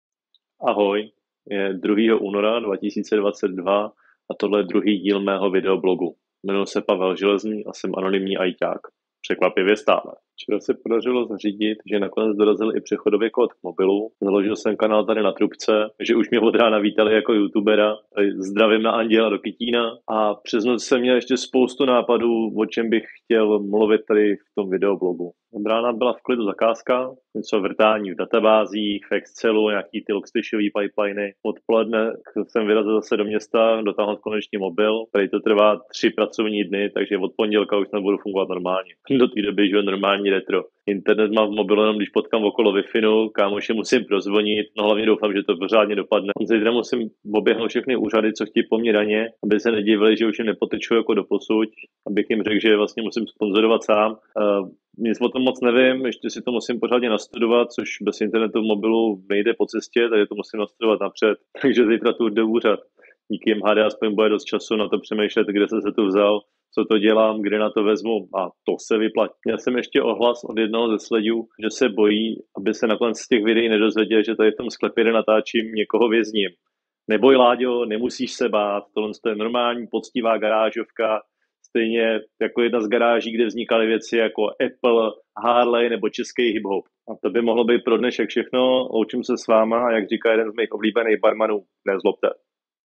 2022-02-02 - Převádím telefon
Přehled událostí všedního dne jednoho naprosto průměrného "boomera" proložená laciným humorem se stále hroznou kamerou, mizerným zvukem a  neumělým střihem. Naštěstí toho nejsou ani dvě minuty.